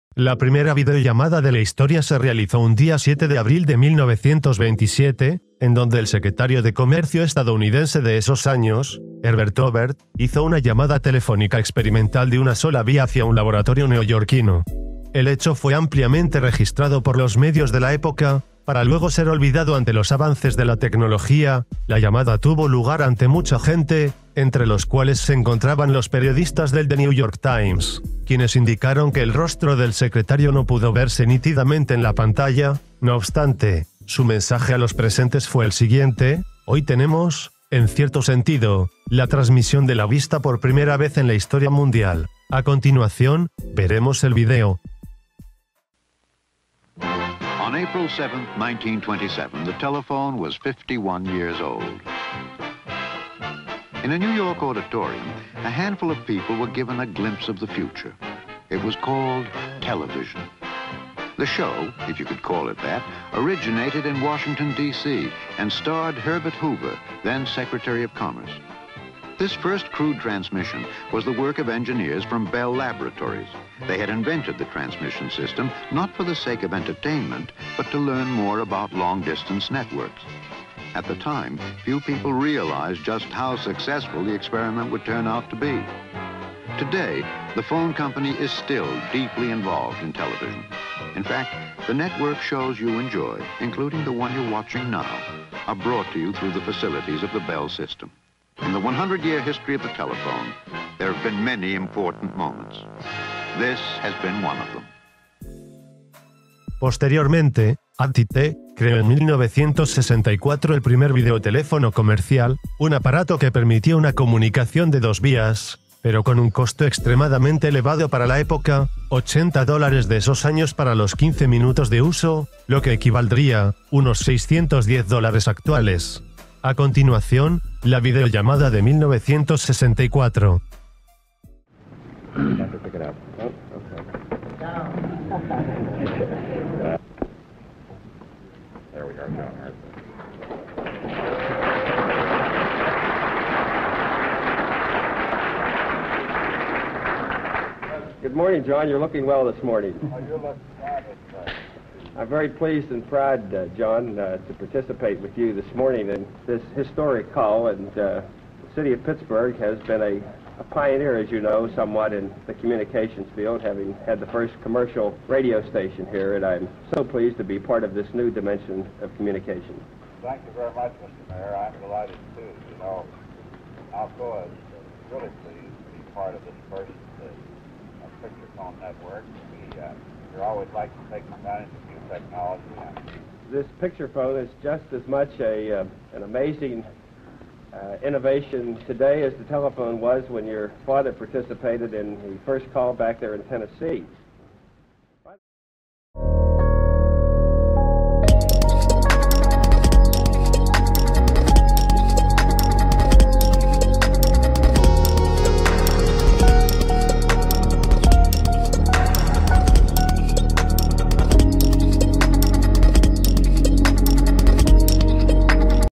📹Primera VIDEOLLAMADA de la HISTORIA
¿Cuándo fue la primera videollamada? ¿Quién lo hizo?

Bienvenido a Jimmy Channel.

NARRACIÓN:
La primera videollamada de la historia se realizó un día 7 de abril de 1927, en donde el Secretario de Comercio estadounidense de esos años, Herbert Hoover, hizo una llamada telefónica experimental de una sola vía hacia un laboratorio neoyorquino.

El hecho fue ampliamente registrado por los medios de la época, para luego ser olvidado ante los avances de la tecnología, la llamada tuvo lugar ante mucha gente, entre los cuales se encontraban los periodistas del The New York Times, quienes indicaron que el rostro del secretario no pudo verse nítidamente en la pantalla, no obstante, su mensaje a los presentes fue el siguiente:

Hoy tenemos, en cierto sentido, la transmisión de la vista por primera vez en la historia mundial.
El mismo medio indica además que los hombres de negocios de la época, además de ciertos representantes de gobierno, antes de la transmisión de voz e imagen, temían que la televisión, aparato que por esos días recién estaba viendo la luz, no fuese un medio comercialmente rentable. Poco sabían lo que se vendría en adelante por ese entonces, claramente.
A continuación, veremos el vídeo:

Posteriormente, AT&T creó en 1964 el primer videoteléfono comercial, un aparato que permitía una comunicación de dos vías, pero con un costo extremadamente elevado para la época: USD $80 de esos años para los 15 minutos de uso, unos USD $610 actuales.

A continuación, el video de este.